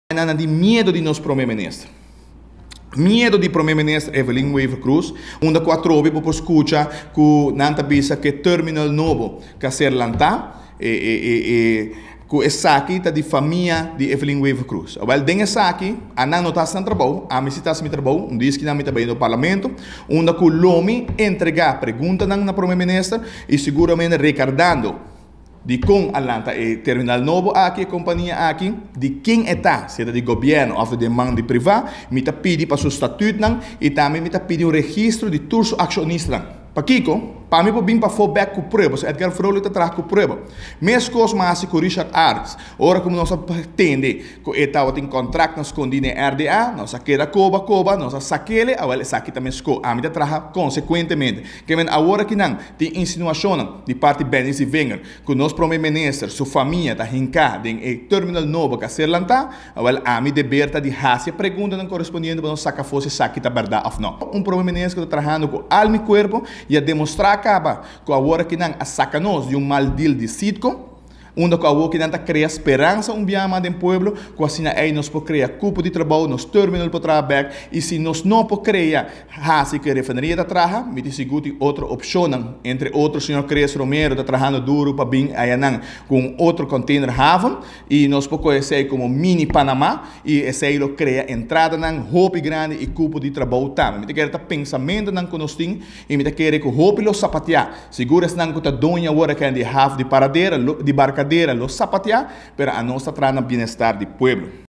Parlamentario Edgar Vrolijk Fraccion di AVP ta demostra cu tin miedo di prome minister
Parlamentario Edgar vrolijk a expresa cu mirando tur e esenarionan cu AVP ta haci, ta mustra claramente cu oposicion tin miedo di Prome Minister Evelyn Wever Croes. Sinembargo grita den pueblo sin prueba ta otro cu si bay parlamento y haci e preguntanan conserni y ricibi acalarion riba tur nan preocupacionnan.